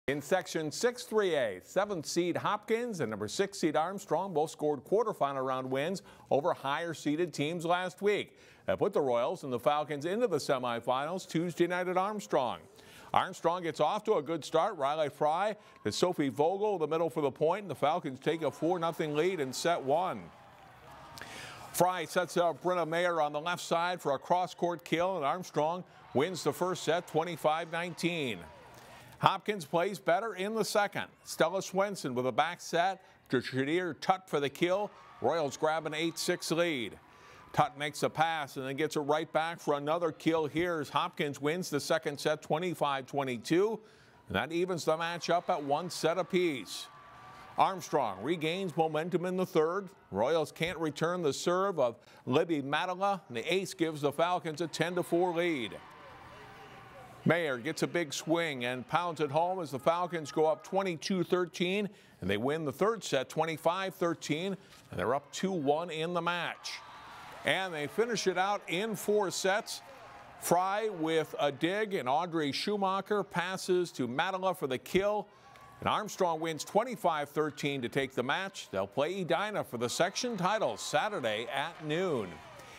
Armstrong Tops Hopkins in 6AAA Volleyball
The Armstrong volleyball team eliminated Hopkins in four sets in the semifinal round of the Section 6AAA tournament. The Falcons defeated the Royals 25-19, 22-25, 25-13 and 25-13. The win puts the Falcons into the section final Saturday, where they'll face top seed Edina.